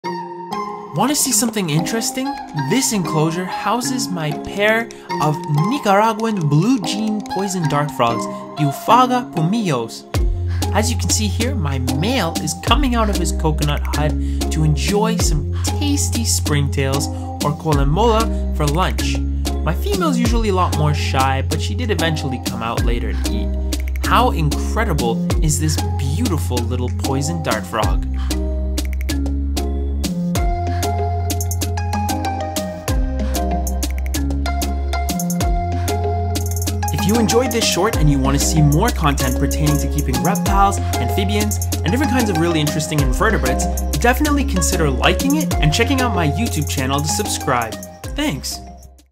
Blue Jeans Poison Dart Frog Eating Springtails (Oophaga pumilio)
My male blue jeans poison dart frog (Oophaga pumilio) eating springtails.